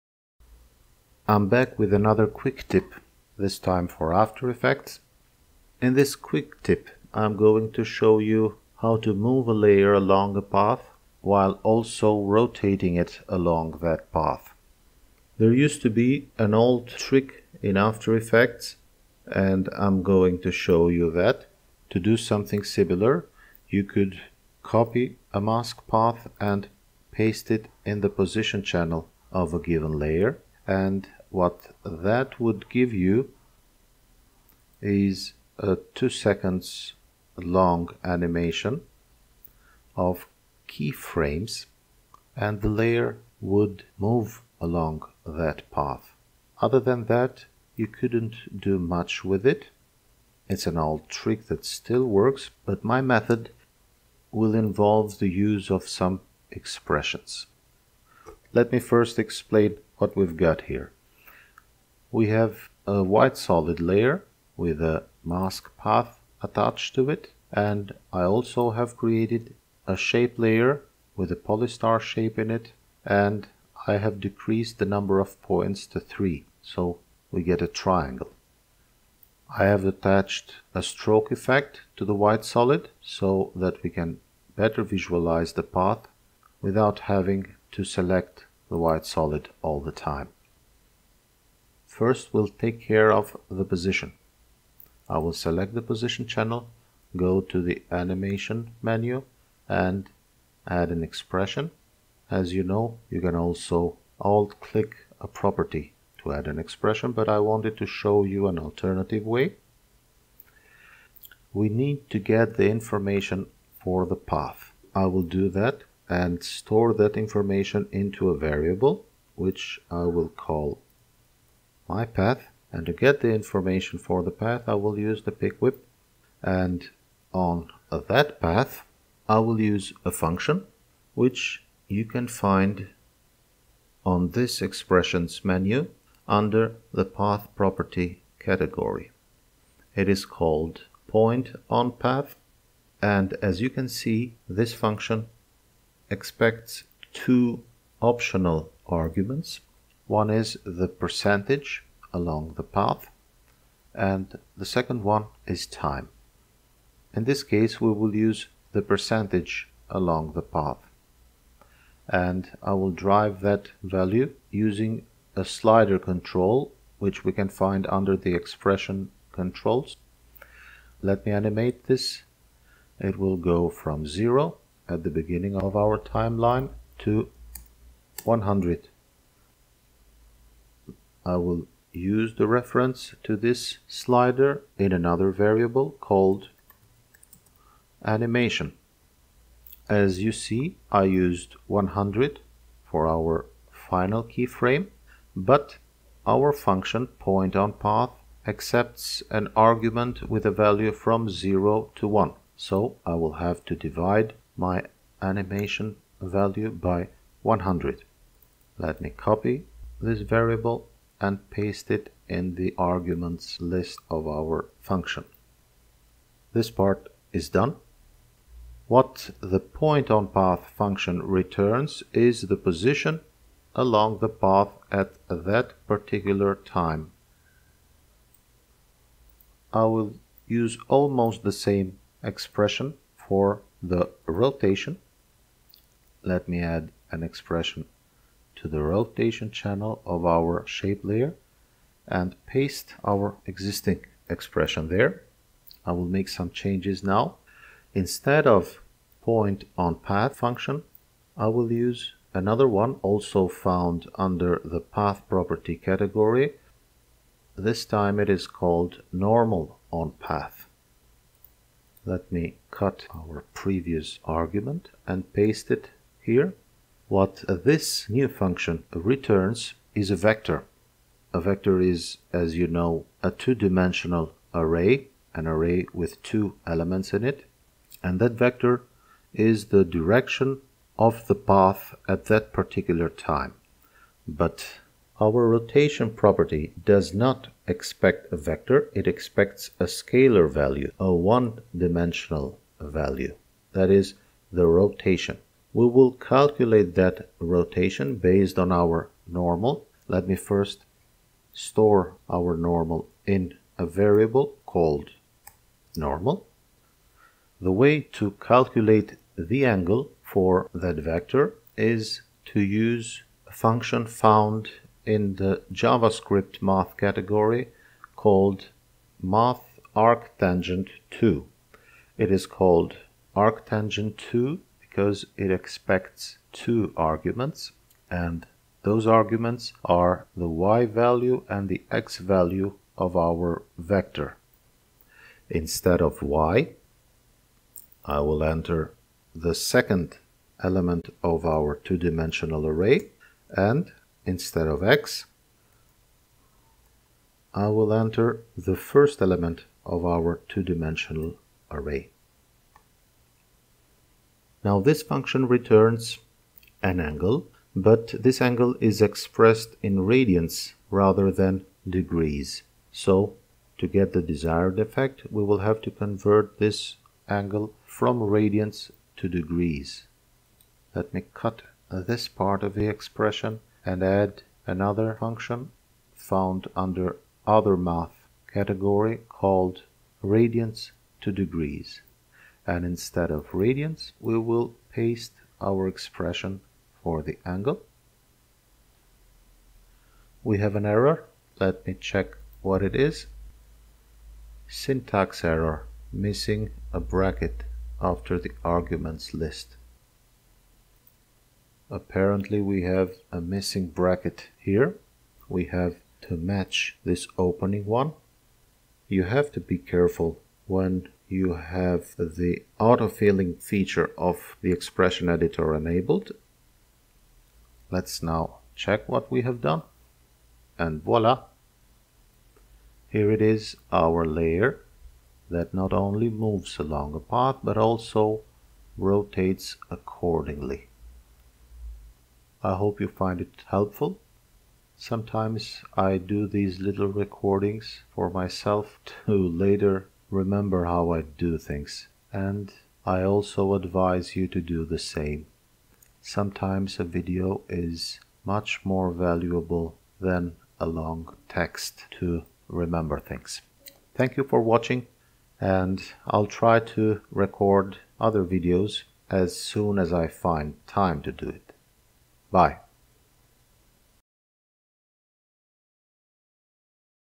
Align And Move A Layer Along A Path In After Effects
I always wanted to know how to do this for regular layers (not just text). Now I do and here I'm sharing it with everyone.